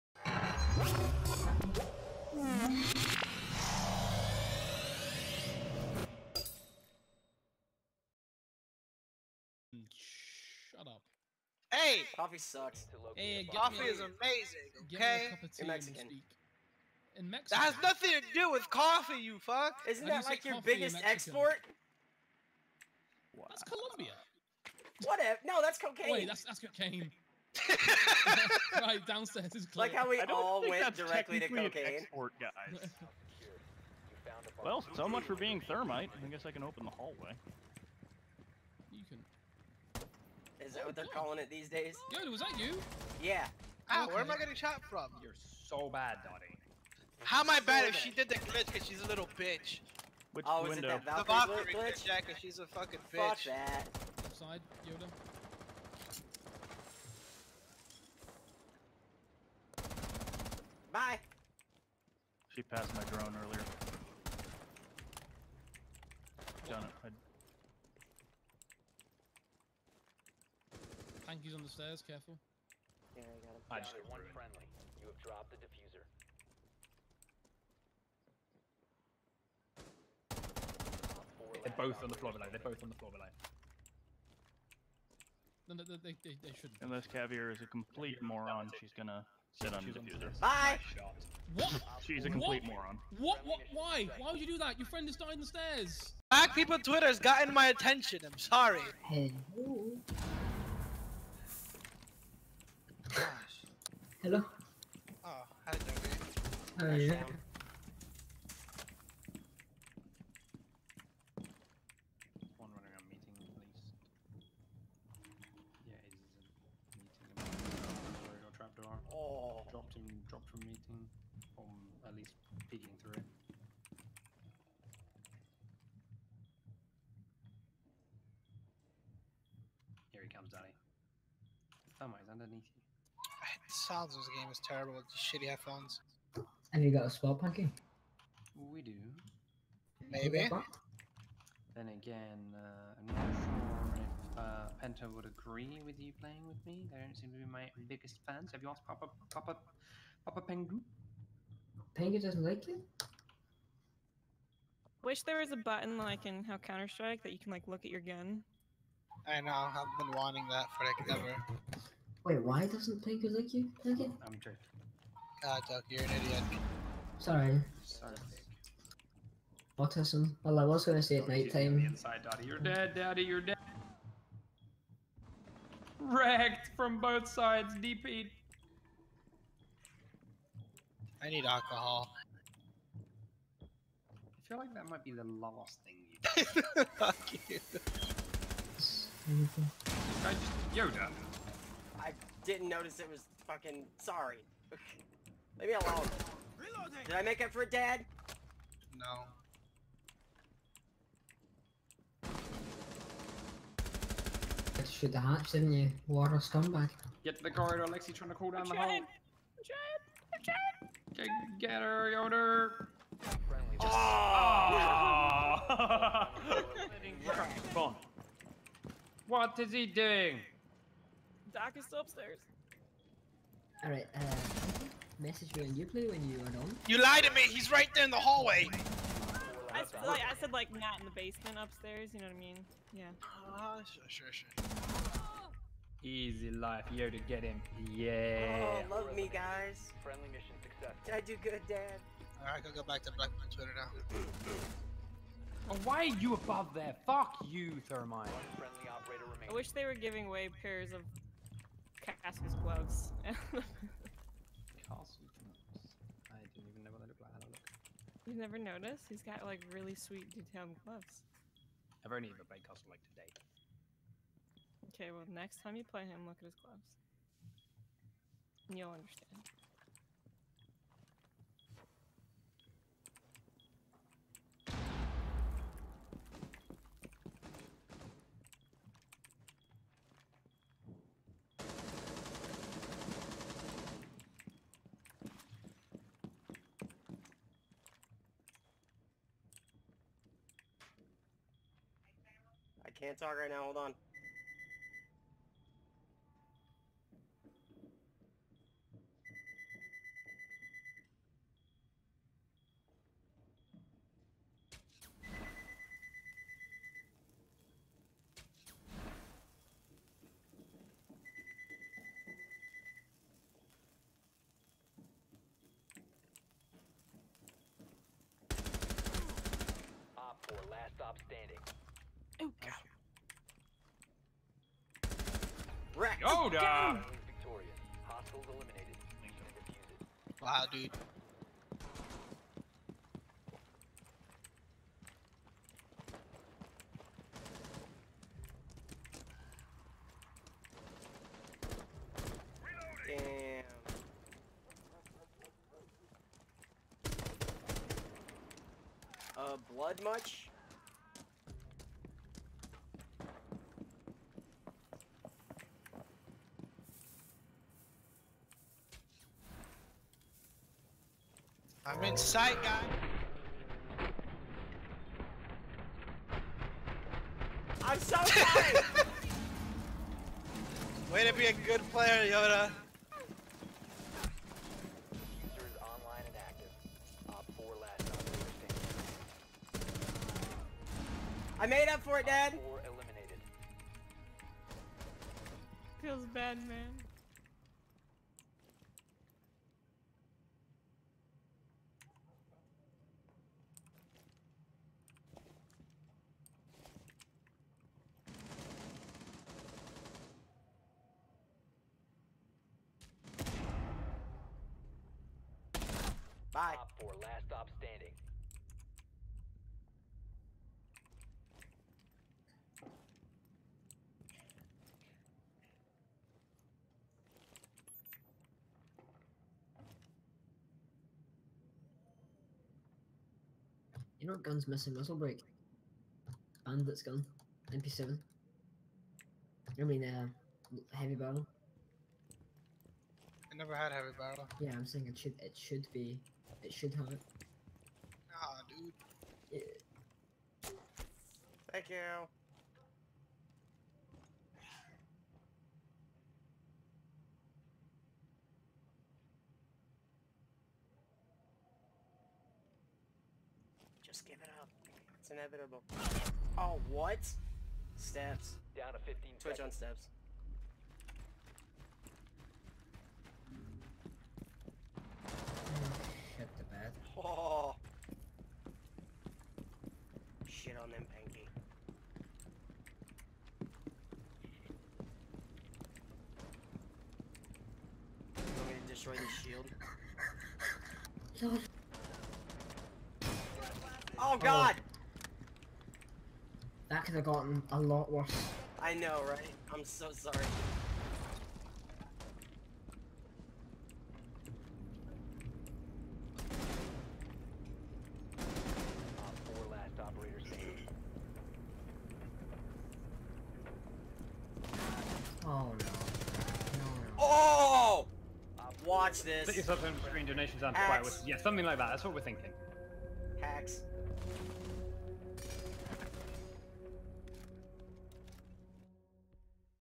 LilLexii FeelsBadMan
Some Ranked game highlights with LilLexii and Panky from yesterday!